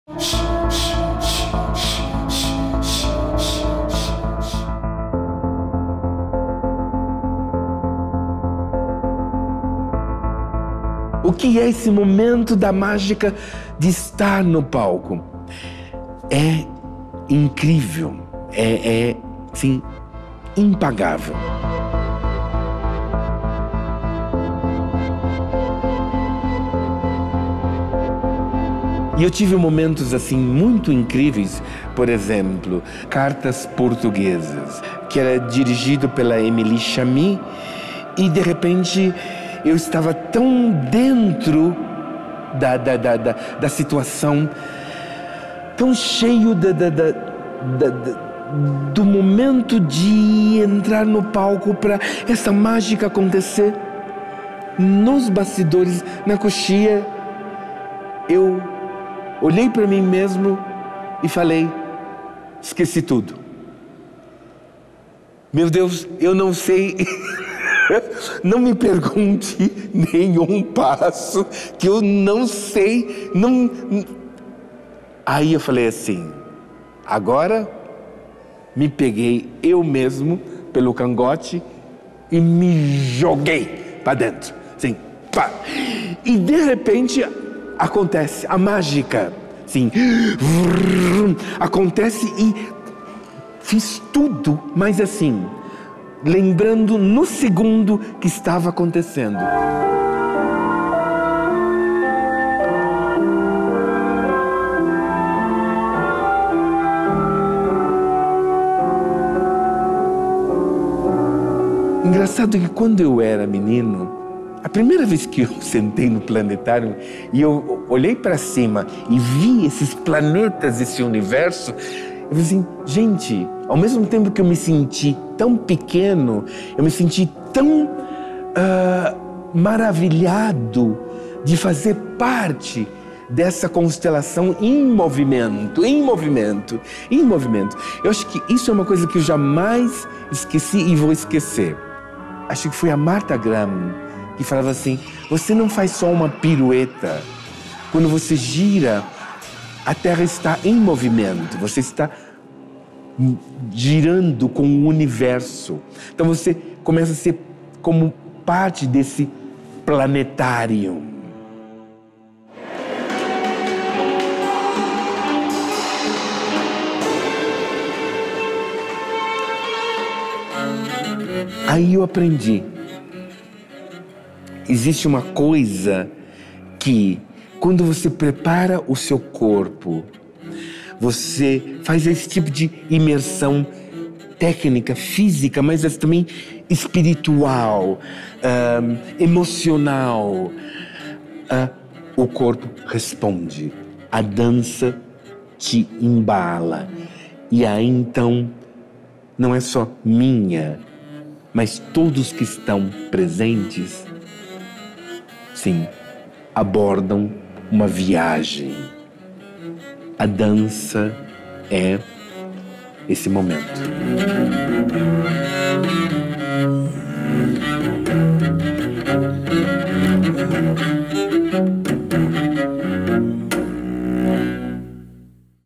HOMENAGEM A ISMAEL IVO, VÍTIMA DA COVID-19
Para o coreógrafo e bailarino Ismael Ivo, ocupar o palco era um momento mágico. O diretor artístico do Balé da Cidade de São Paulo morreu ontem, aos 66 anos, vítima da Covid-19. Ismael dirigiu o Festival de Dança da Bienal de Veneza e também o Teatro Nacional Alemão, em Weimar.

Assista aqui ao quadro Primeira Vez, gravado com Ismael Ivo em 2017, no Theatro Municipal de São Paulo. Na ocasião, o dançarino relembrou uma das estreias mais marcantes de sua trajetória nos palcos: o espetáculo “Cartas Portuguesas", concebido por Emilie Chamie em 1978.